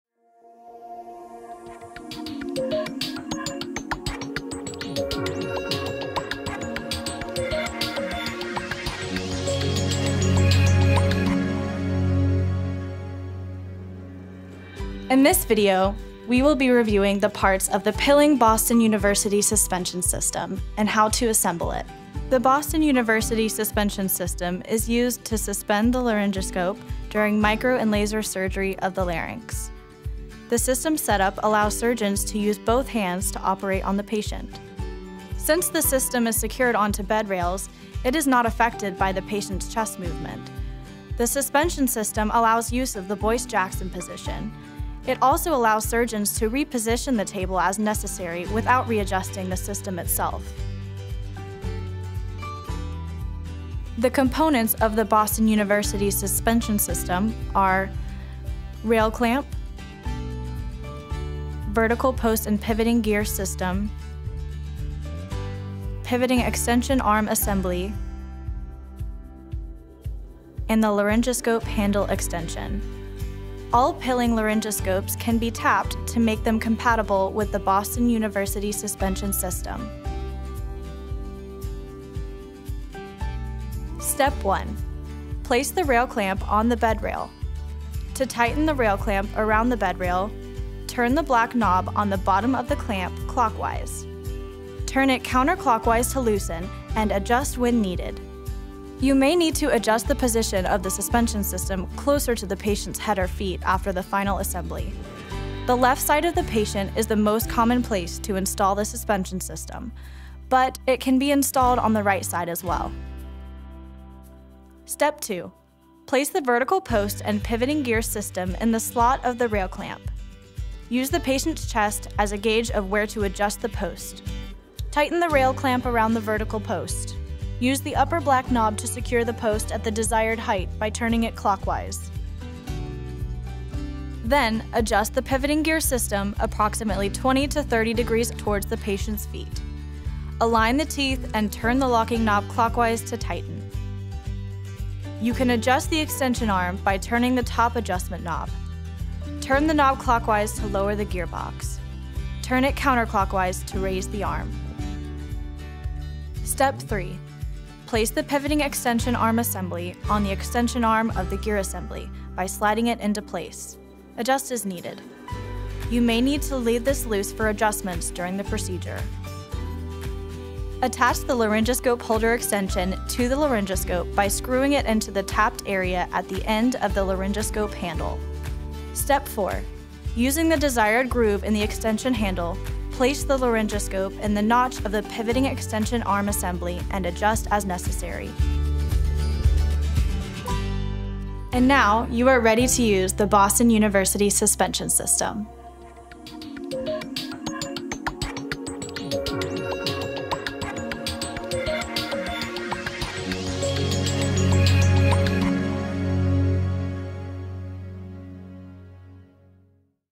Pilling Boston University Suspension System Assembly In-Service
This video provides a step-by-step demonstration of how to assemble the Pilling™ Boston University Suspension System.